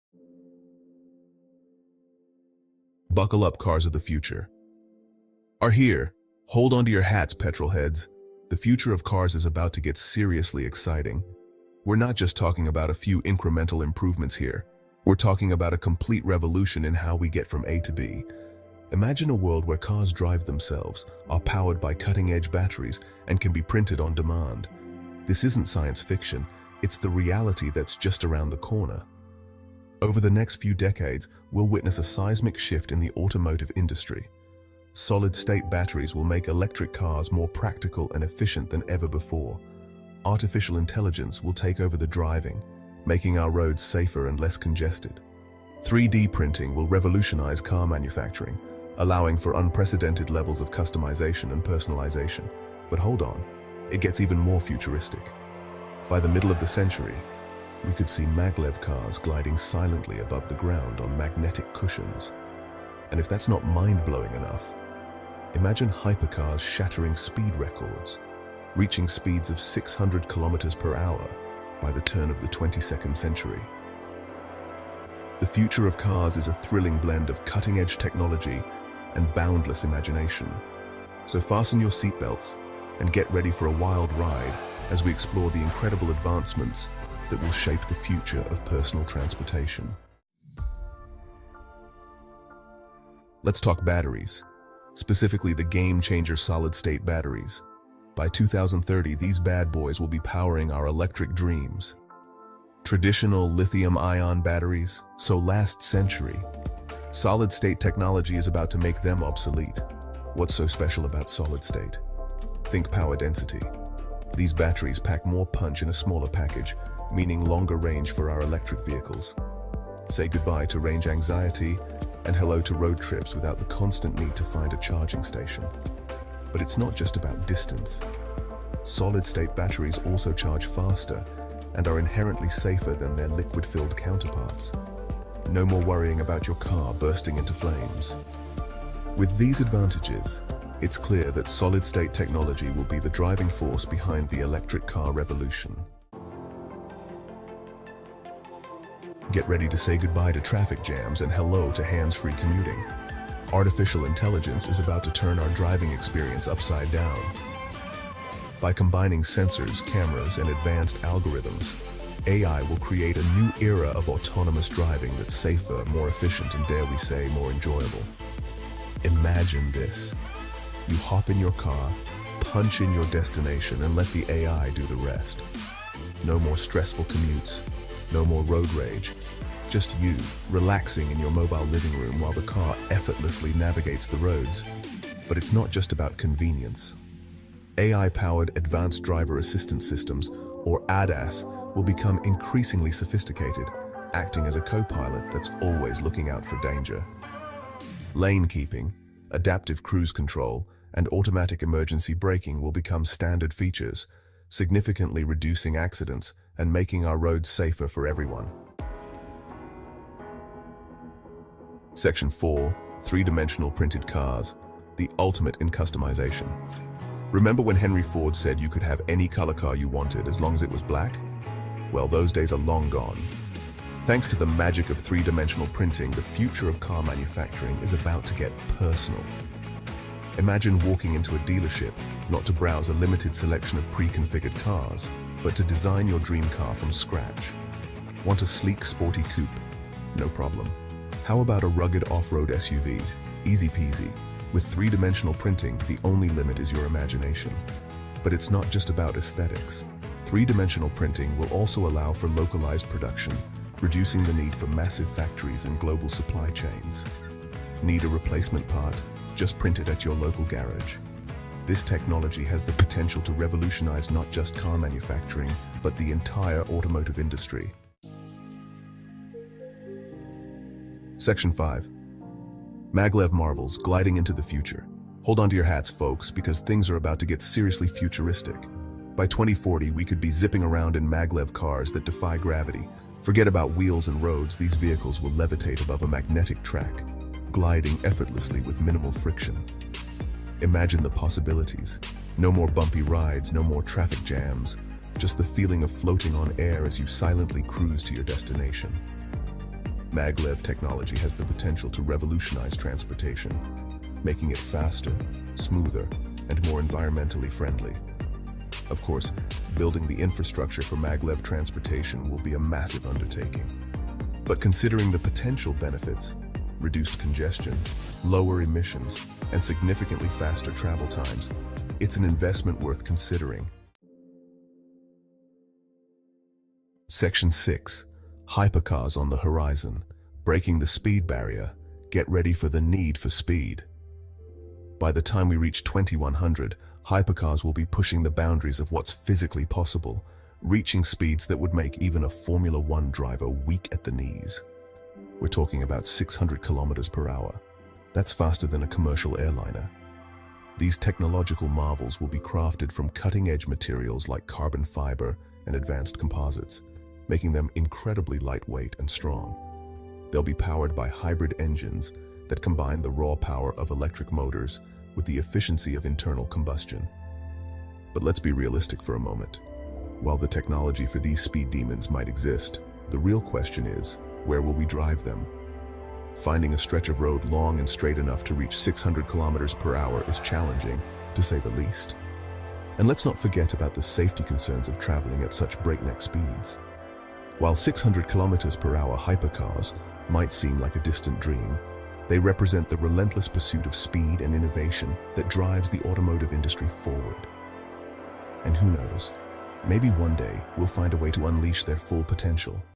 Future Cars: 600km/h hypercars, Solid-State Batteries and Maglev by 2100!!
In this video, we explore the cutting-edge innovations that could redefine the automotive landscape by the end of the century. Imagine hypercars capable of reaching mind-boggling speeds of 600km/h, pushing the limits of engineering and performance. We'll also delve into the game-changing potential of solid-state batteries, set to revolutionise energy storage with faster charging times, greater range, and enhanced safety. Finally, we’ll discuss the future of transportation with Maglev technology, promising levitating cars and super-smooth, frictionless travel. From mind-blowing speeds to breakthrough energy and propulsion systems, the future of cars looks more exciting than ever!